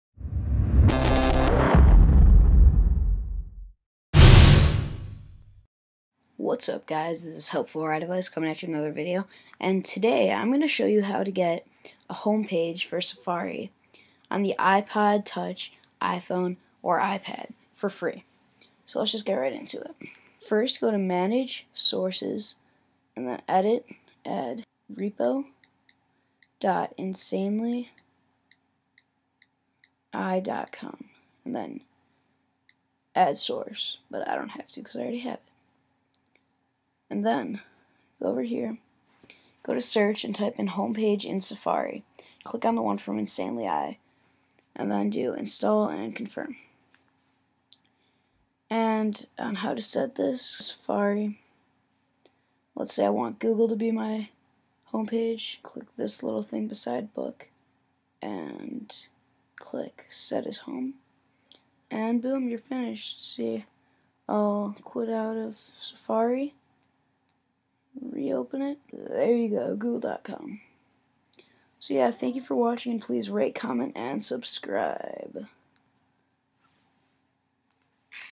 How to Get a Home Page on Safari - iPod Touch/iPhone/iPad
Hey Guys! This video shows you how to get a home page on safari for free!
I'm currently taking video requests!
Thanks!
-Help4iDevice